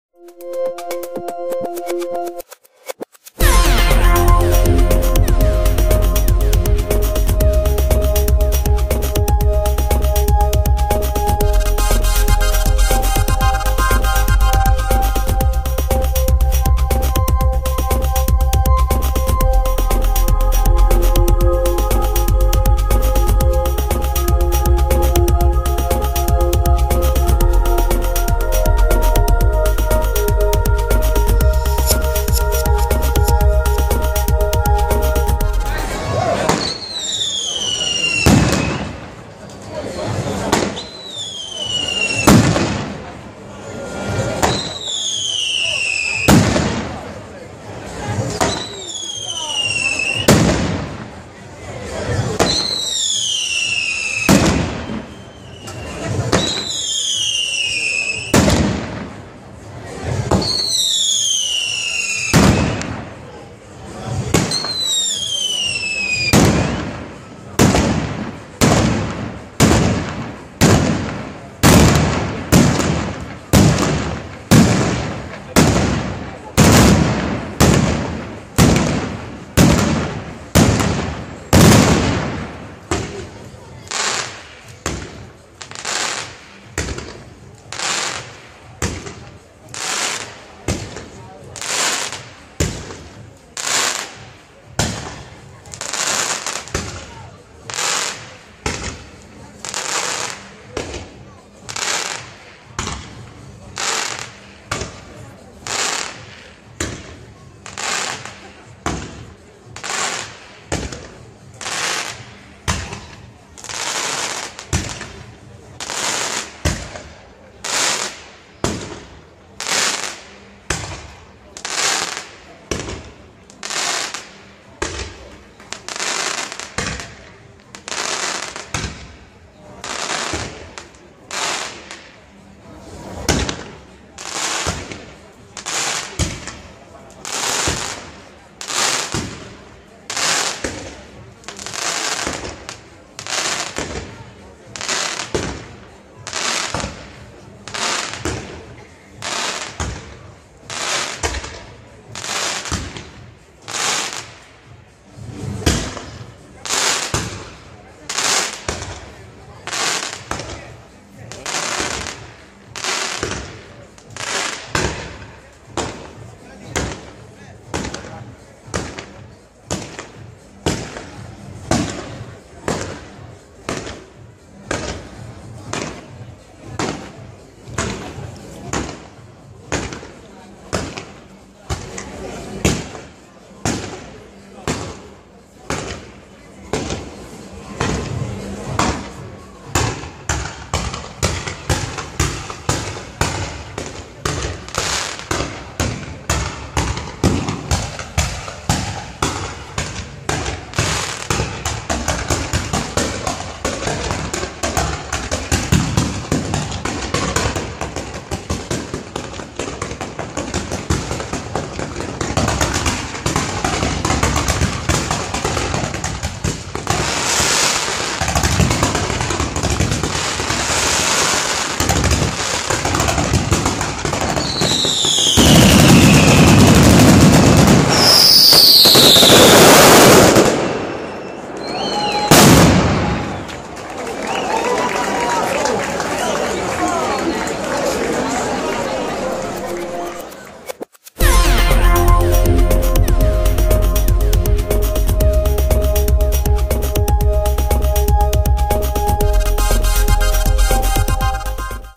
BELLONA (Caserta) - Pirotecnica SICILIANO (Batteria 2016)
BELLONA (Caserta) - 31 Marzo 2016
Festeggiamenti in onore di Maria Ss. di Gerusalemme

Batteria sequenziale a cura di
Pirotecnica SICILIANO da Vitulazio (Ce)